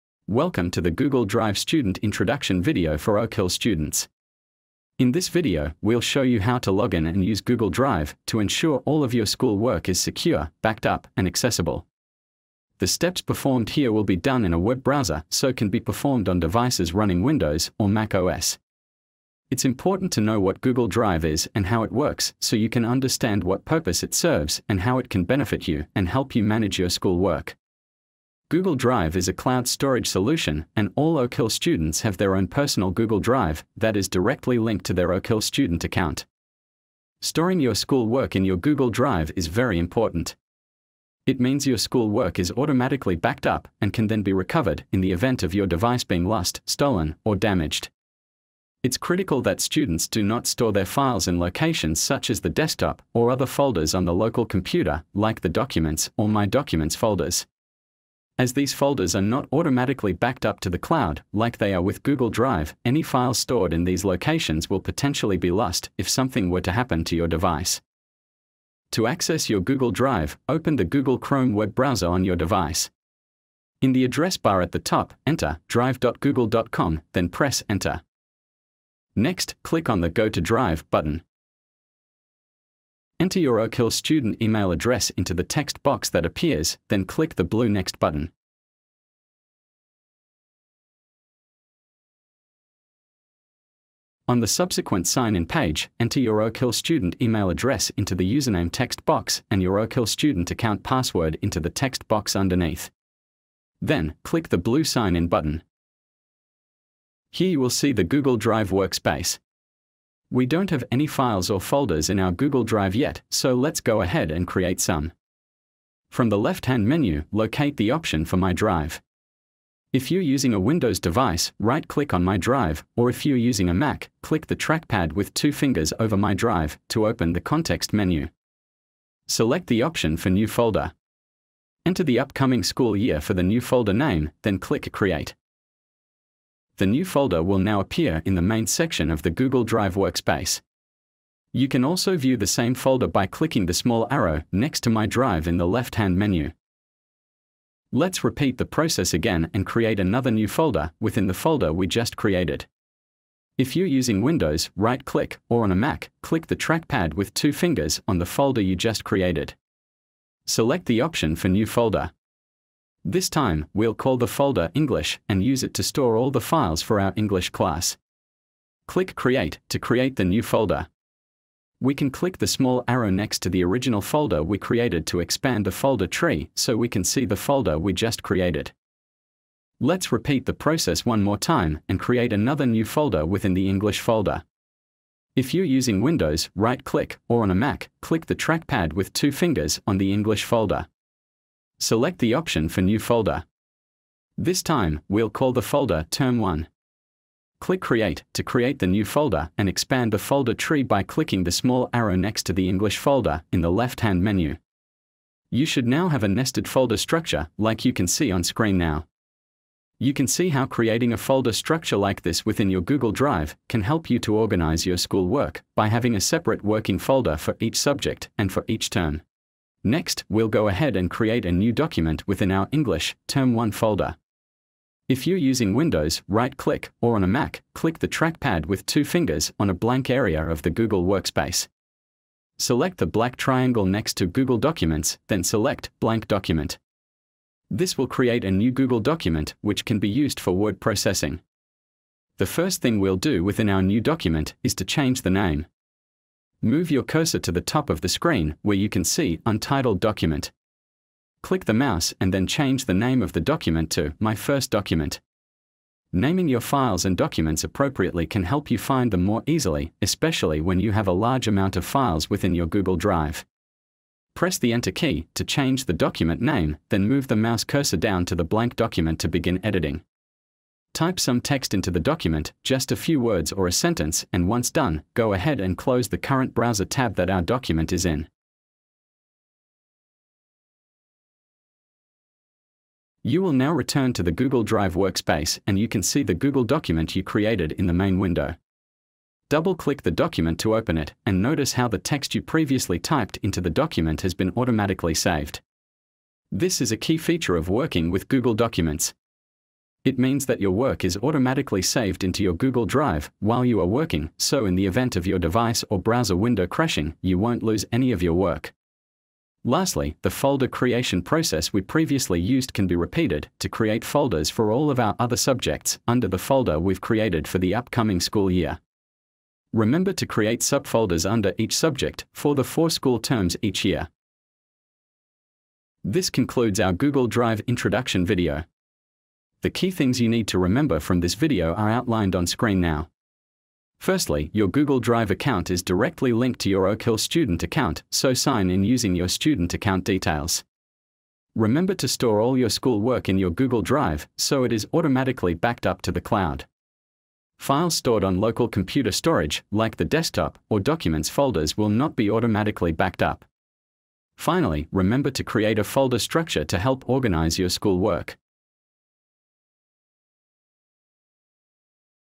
Google Drive Student Introduction and Setup
How to install and configure Google Drive, for Oakhill College students.